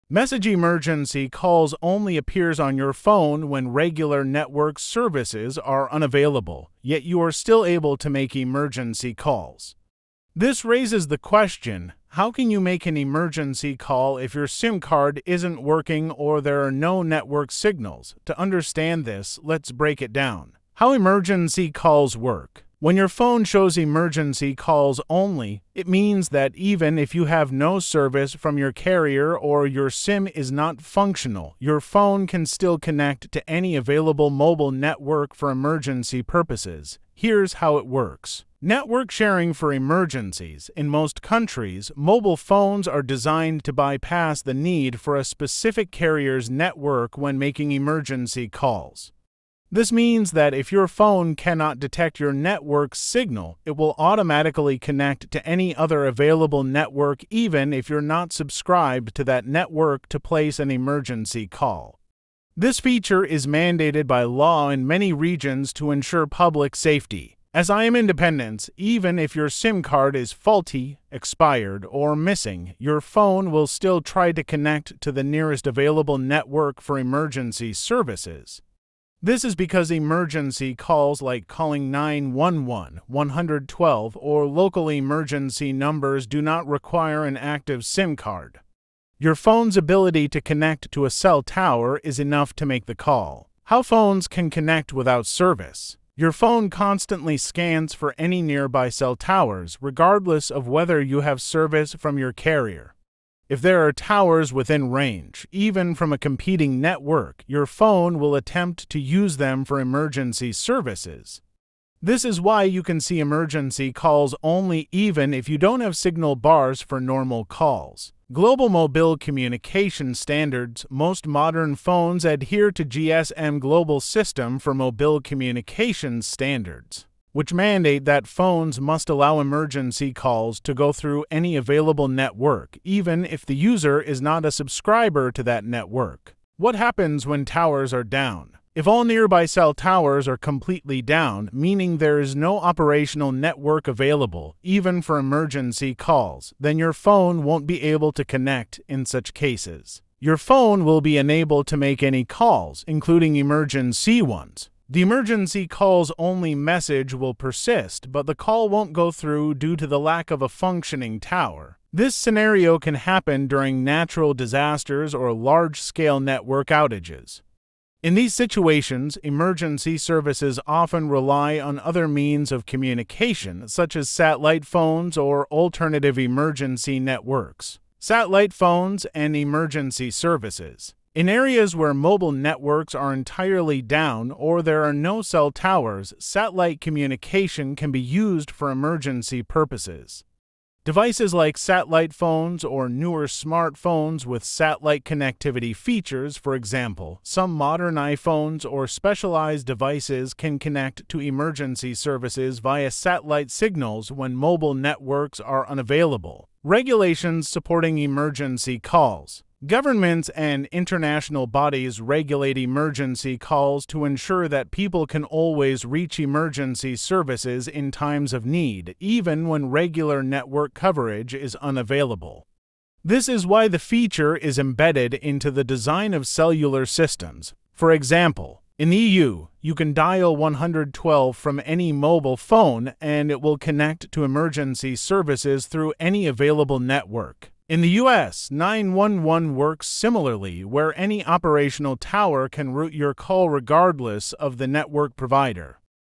How Emergency Calls Work Without Signal or SIM: Explained!
How Emergency Calls Work Without Signal or SIM: Explained! @anwarjawed